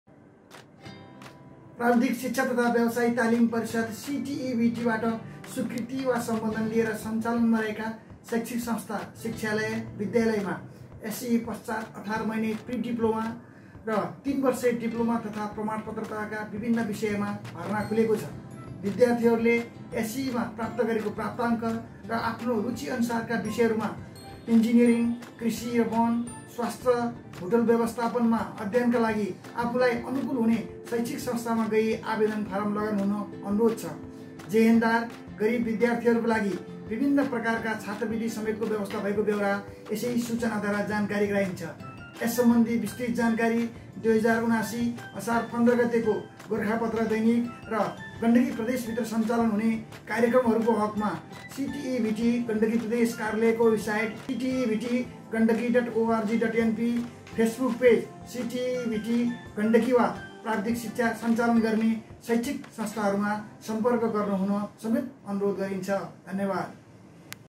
Pramod Bhakta Acharya | Director | CTEVT Gandaki Province | Pokhara Technical School
CTEVT Admission Information for 2079/080 by Pramod Bhakta Acharya, Director, CTEVT Gandaki Province